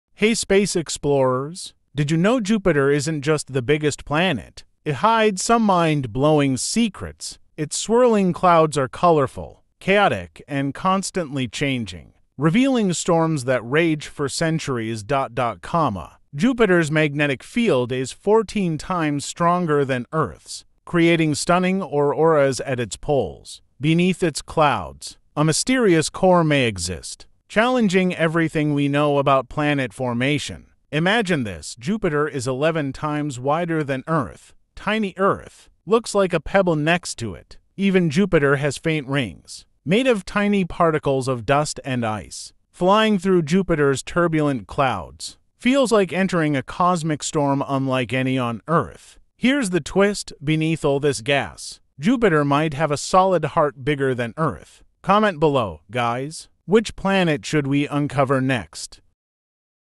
Jupiter: The Giant King with Deadly Secrets!
Jupiter isn’t just the biggest planet in our solar system—it’s a world of terrifying mysteries! 🌌
Its Great Red Spot is a storm that’s been raging for centuries, and its magnetic field is so powerful it could tear apart spacecraft that come too close. ⚡🌀
But here’s the twist… scientists believe Jupiter might actually be Earth’s protector, shielding us from deadly cosmic impacts. 🌍✨